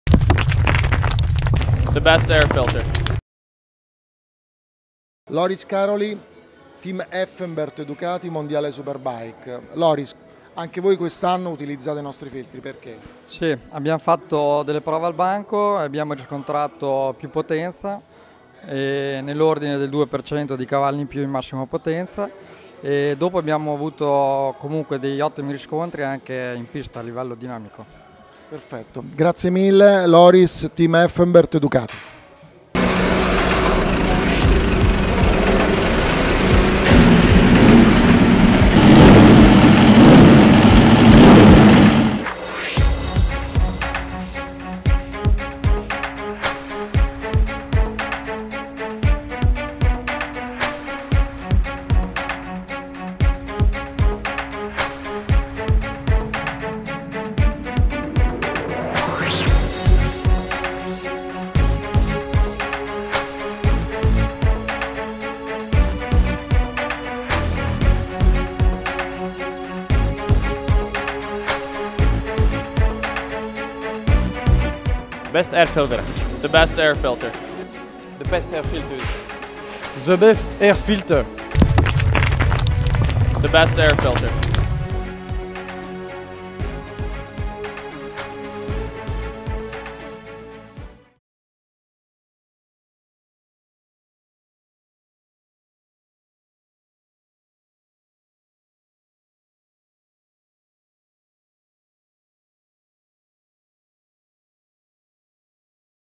Sprint Filter Effenbert Liberty Racing
The World Superbike Team Effenbert Liberty racing uses Sprint Filter P08 on their bikes. Jacub Smrz, Sylvain Guintoli, Maxime Berger, Brett McCormick